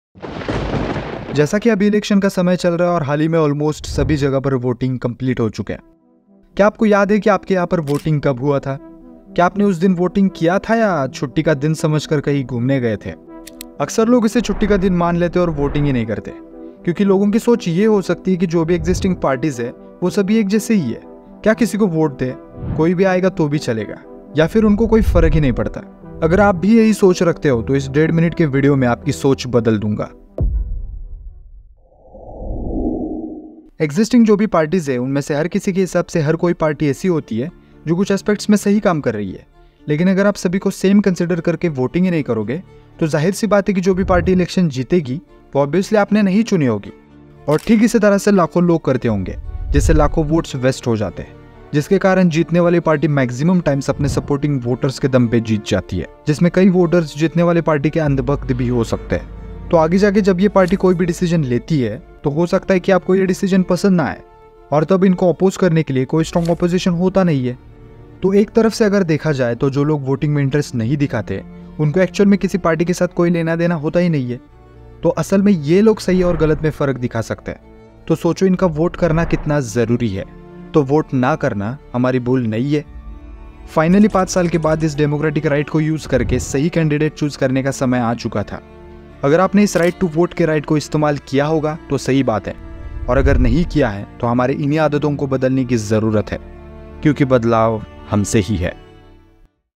Why EVERY VOTE Is Important 🤔 || Election Results..
The power to vote for your country is an important part of a democratic country like India.
In a democracy with compulsory voting, each vote is equal, every voice is equal.

By voting, you are part of the decision; you are making a choice about how your community,
state, or country functions. If you don’t vote, you are relying on someone else to choose for you.

Make sure you are involved in decisions about your community by voting at every election.
Someone struggled for your "Right to vote". Use it..!